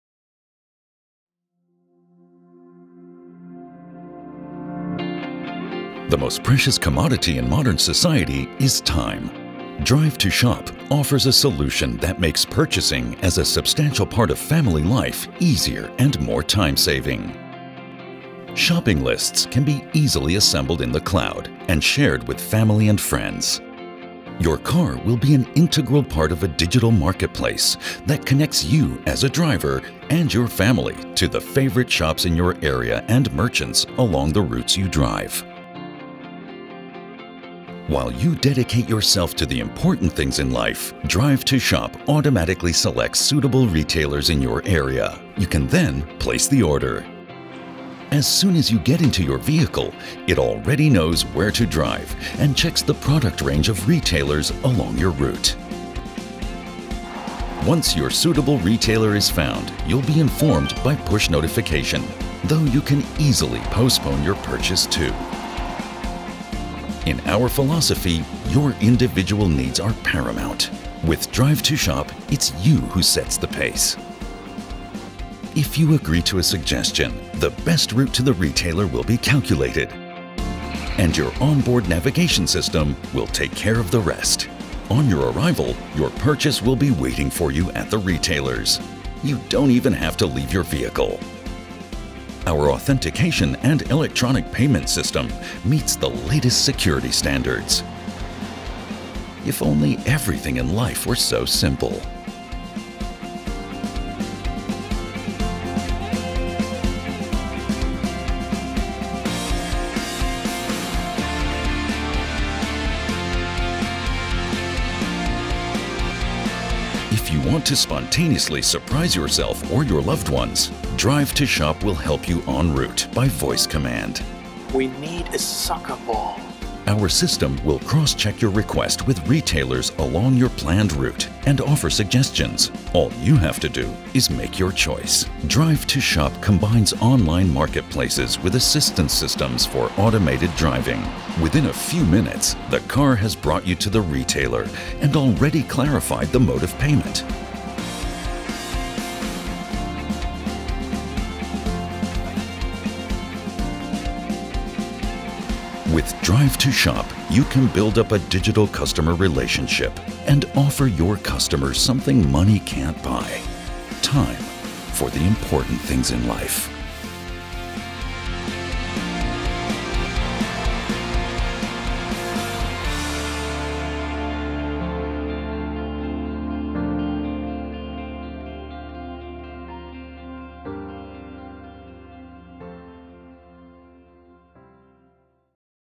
IAV - Drive2Shop: A digital marketplace for everyone (CES 2018)
Time is the most valuable asset in our society, and in no other place do people spend as much time doing nothing as in the car. Autonomous driving may resolve this contradiction in the future - but why should it not already be possible to spend time in the car efficiently and safely?

Drive2Shop brings the solution: the automobile as a digital marketplace, where drivers and their families are connected with the shops around them and can make purchases easily.

This is not just beneficial for customers, who will save precious time. Businesses can also get in contact with the customer before purchase, while automobile manufacturers can build permanent digital relationships with their customers - which is completely new in terms of quality and frequency. Long-term benefits to society as a whole: Drive2Shop makes cities more liveable and protects the environment.

How it works? Simple. Intuitive. And above all: reliable. Drive2Shop turns the car into an assistant in everyday life which networks the driver with their environment and enables them to handle everyday tasks.

The system aligns the offerings of local businesses with the wishes of the customer, which are collected in the cloud in advance or spontaneously, by voice input, during the ride. If there is a match, the driver is given the best possible proposal.

The driver can order the desired product and collect it right away; the car navigates the driver directly to the shop. Payment is made when the customer arrives - it’s very straightforward via a secure transaction from the vehicle. The customer picks up their order by Click & Collect, without having to leave the car.

The driver can either operate the system via voice input or touch. It is constantly available (provided there is an adequate internet connection), both in the vehicle as well as via an app on smartphones or tablets.
The company IAV Automotive Engineering is responsible for the visionary ideas driving the development of Drive2Shop and technical know–how for the automotive industry. Concardis, the leading payment service provider in Europe, guarantees secure and easy payment processing.